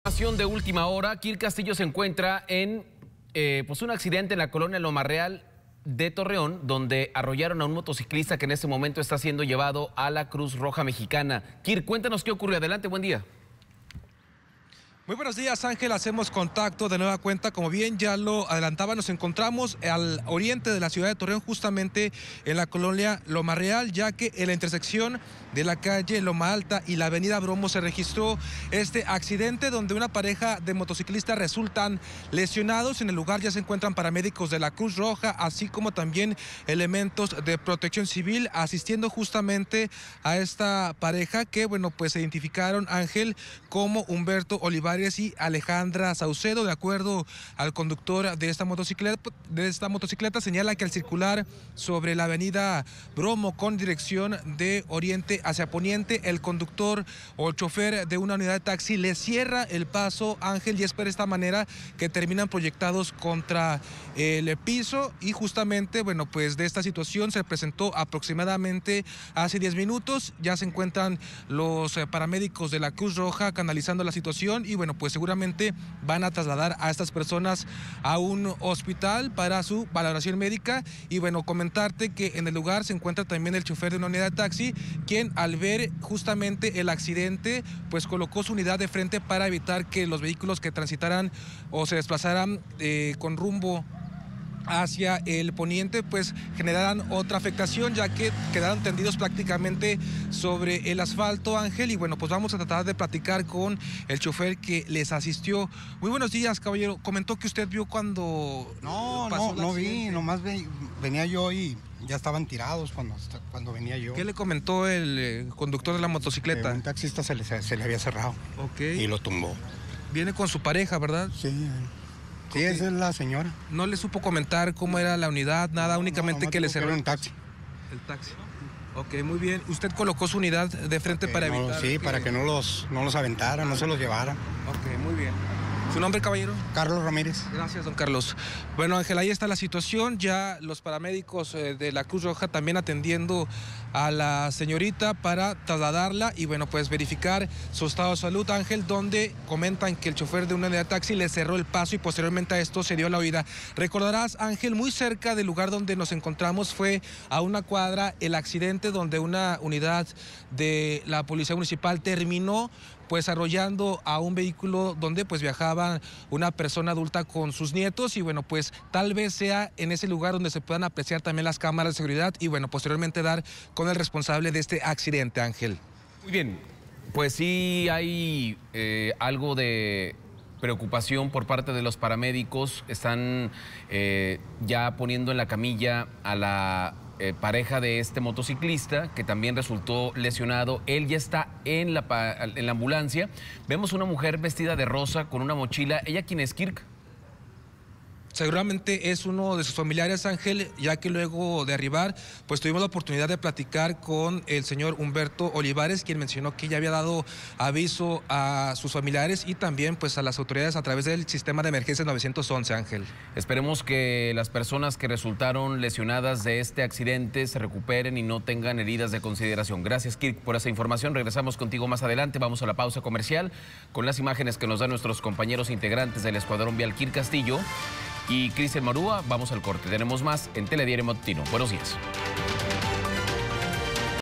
Un hombre y su pareja fueron arrollados en su motocicleta
En la colonia Loma Real de Torreón, una pareja resultó lesionada tras ser arrollada cuando viajaban en una motocicleta.